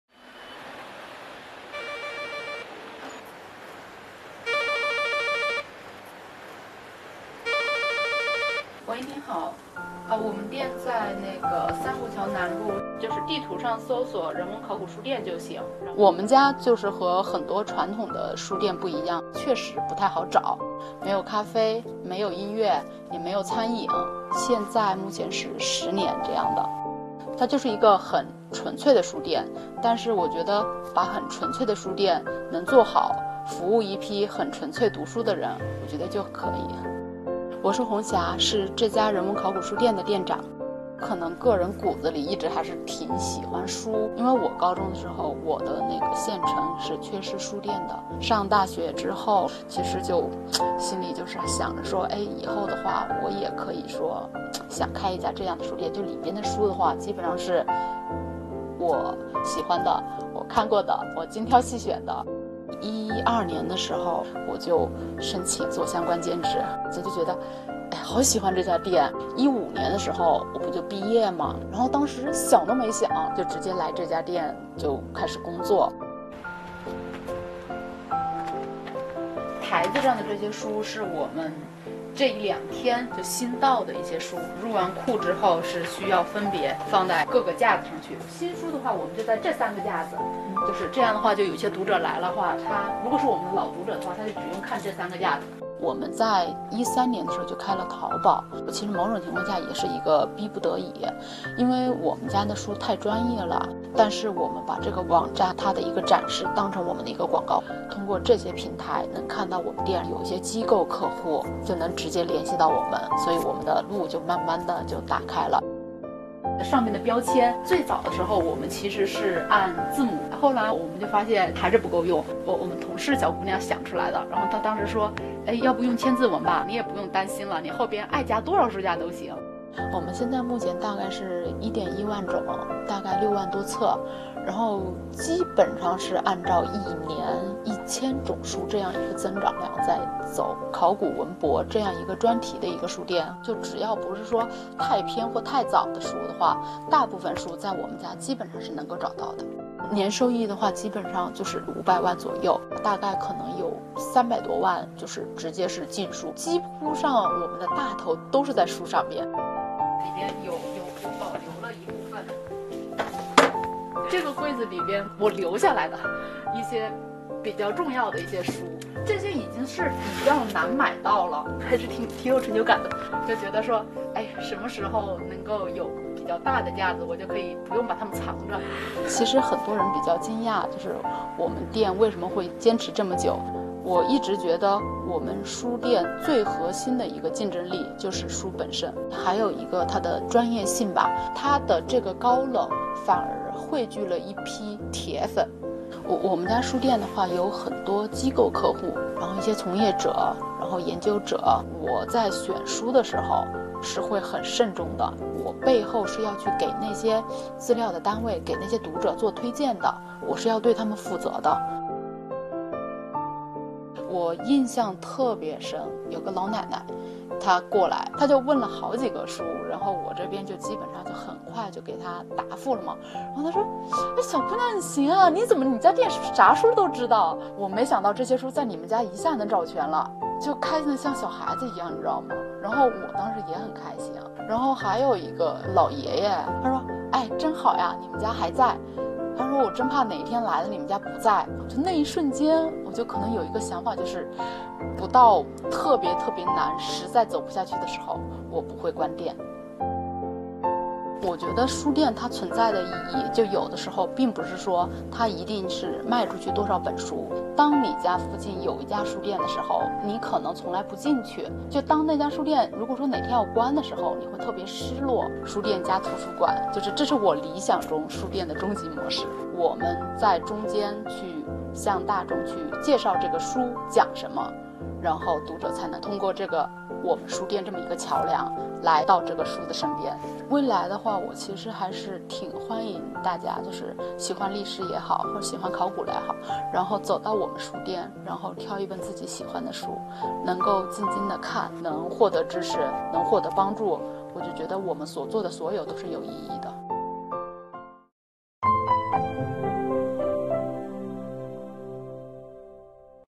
不卖咖啡文创为何能年入五百万？这家“冷门”书店店长谈吸粉秘籍
在北京海淀区，有这样一家特别的书店。没有咖啡音乐和文艺的装潢，书店经营的大多为考古、文博领域的专业书籍。但就是这样一家“高冷”的书店，却得到了一众考古文博界“大拿”的青睐，存活了近十年，年收益目前可达五百万元。

        在实体书店日渐式微的今天，这家看似受众较窄的书店究竟有何吸粉秘籍？又是如何一步步走到了现在？